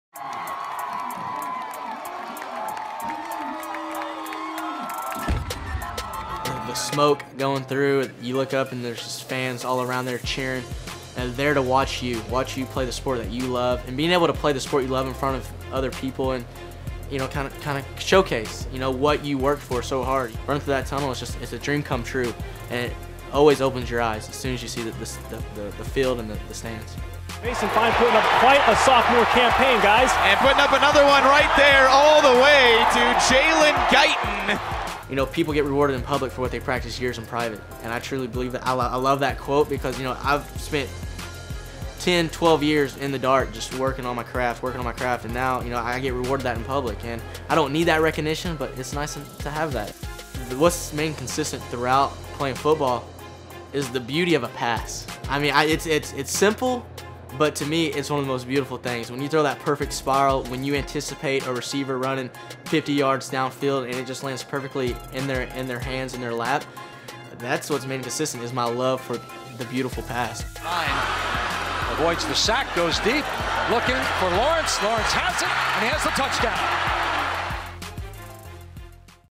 Mason Fine: The Beauty of the Perfect Pass
“People get rewarded in public for what they practice for years in private.”

For North Texas quarterback Mason Fine, taking snaps for the Mean Green is the culmination of years of hard work.